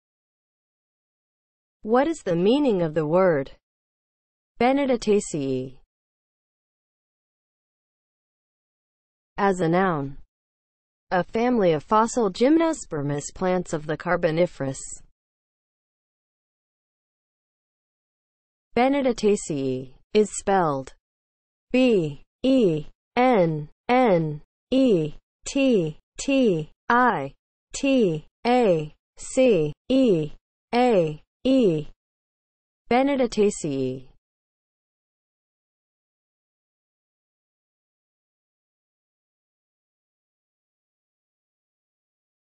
What is the meaning of the word BENNETTITACEAE?
Definition and spelling of the word BENNETTITACEAE.

(00:00) Intro
(00:08) As a noun
(00:17) Spelling

As a noun (00:08):
• a family of fossil gymnospermous plants of the Carboniferous